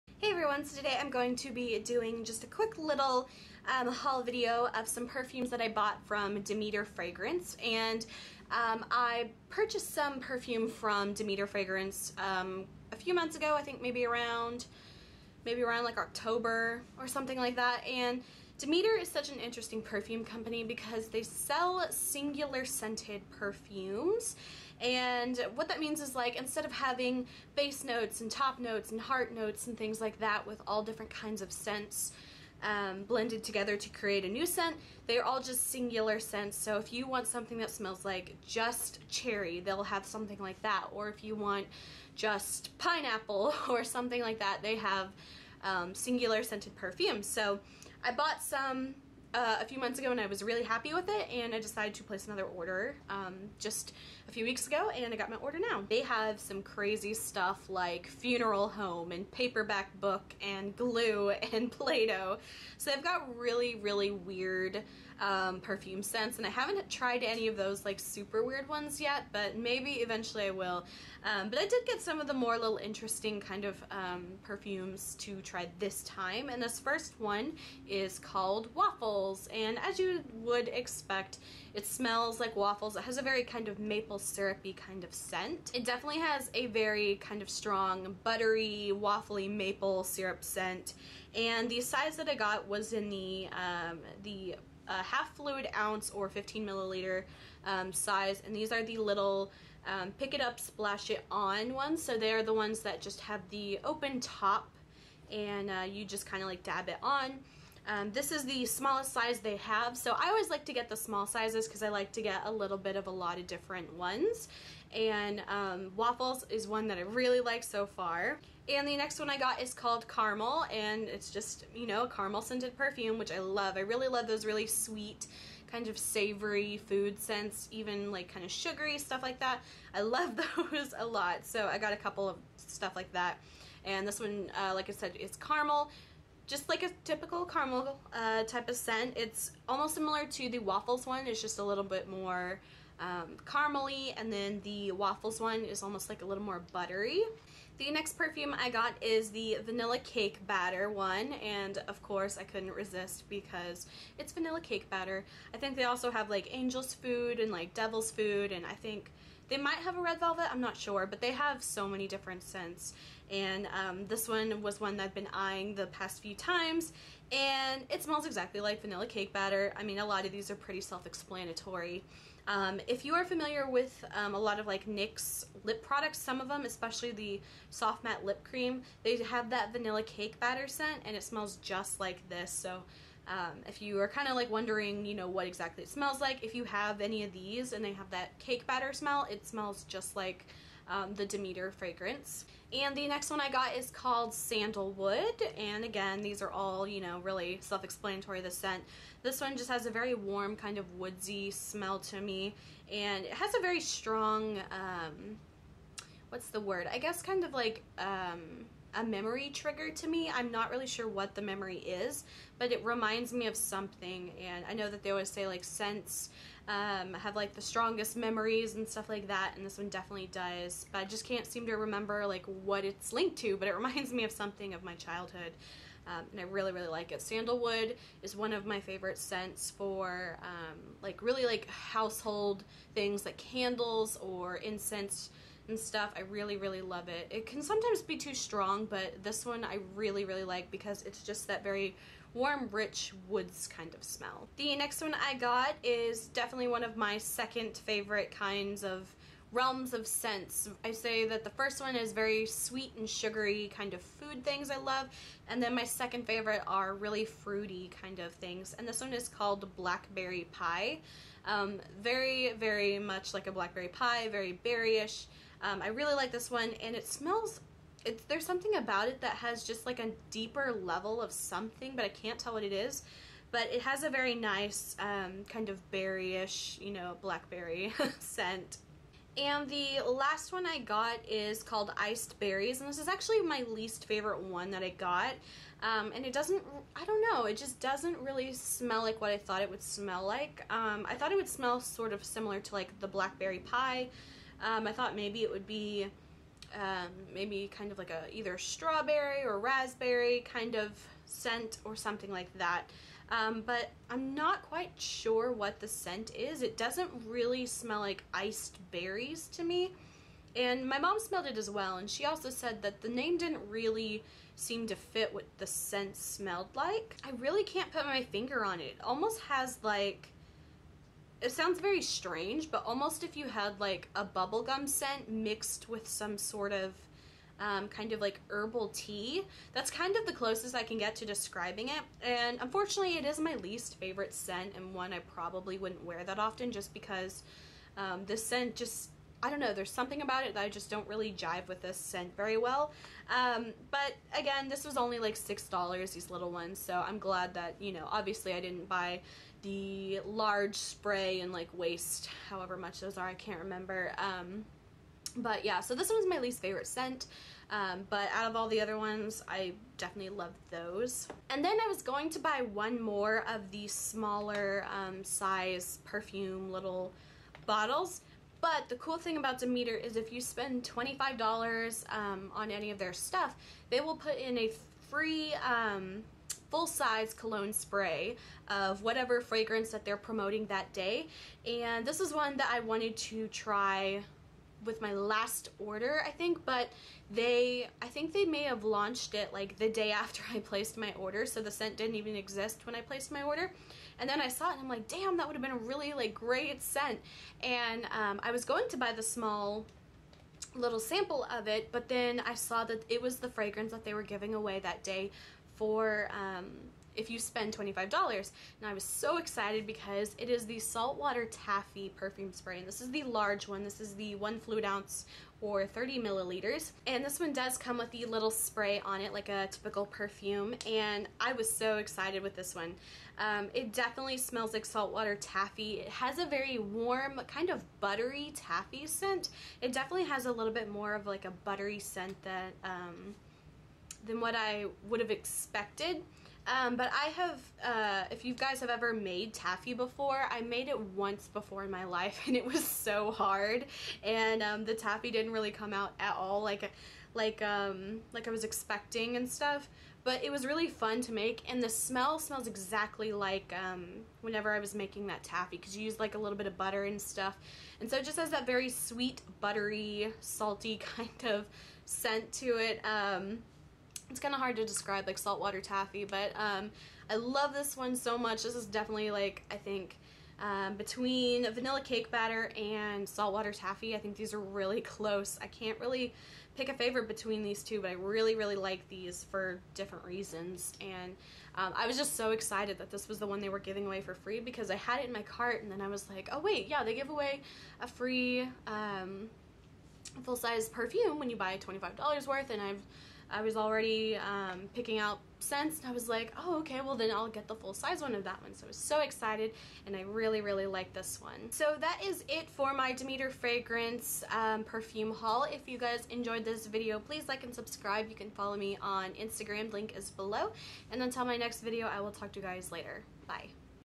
Demeter Fragrance Perfume Haul
Hey everyone! This is a video of my second haul from Demeter Fragrance Library.

Products mentioned
Demeter Waffles
Demeter Caramel
Demeter Vanilla Cake Batter
Demeter Sandalwood
Demeter Blackberry Pie
Demeter Iced Berries
Demeter Saltwater Taffy

I paid for all these products and all opinions are my own.